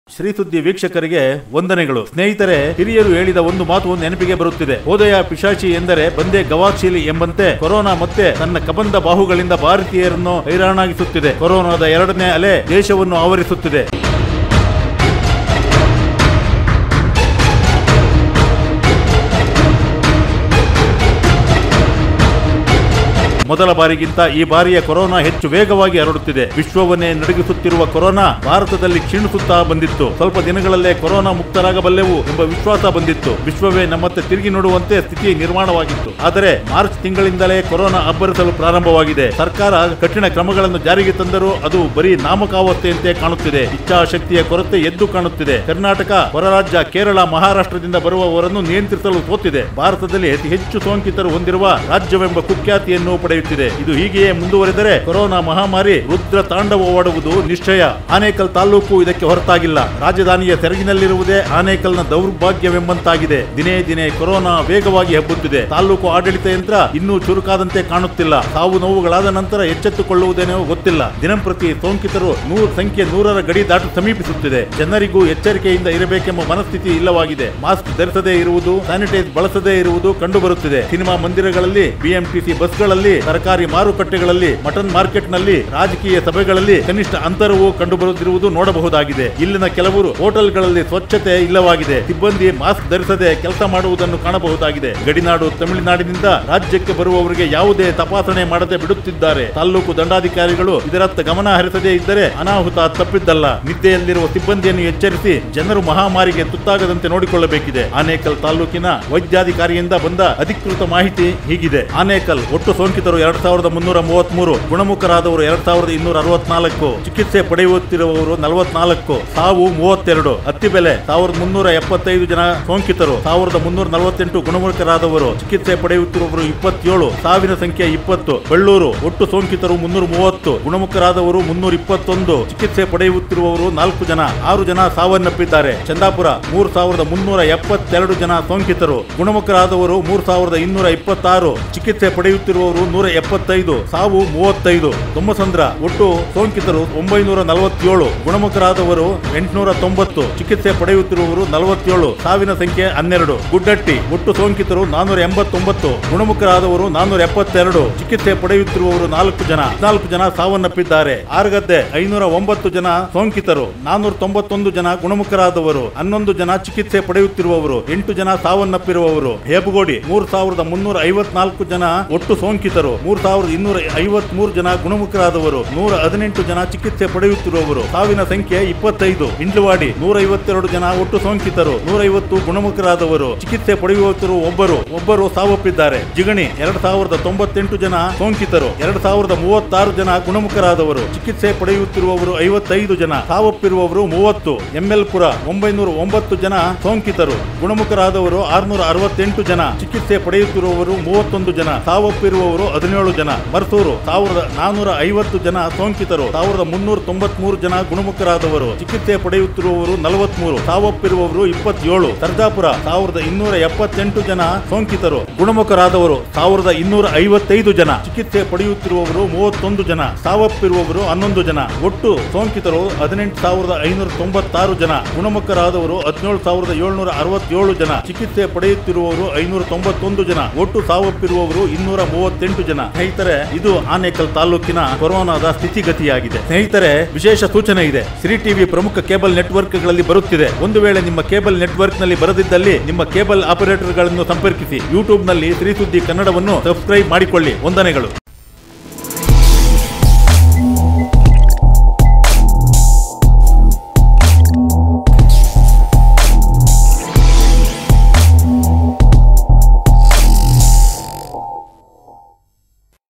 ಎಚ್ಚರ ಆನೇಕಲ್ ತಾಲೂಕಿನಾದ್ಯಂತ ಸೋಂಕಿತರ ಸಾವಿನ ಸಂಖ್ಯೆ 238 ಕ್ಕೆ ಏರಿಕೆ ತಾಲೂಕಿನ ಎಲ್ಲ ಊರುಗಳ ಮಾಹಿತಿ ನೋಡಿ anekal
Welcome to ♥ SRI SUDDI KANNADA™ ♥ Kannada YouTube Channel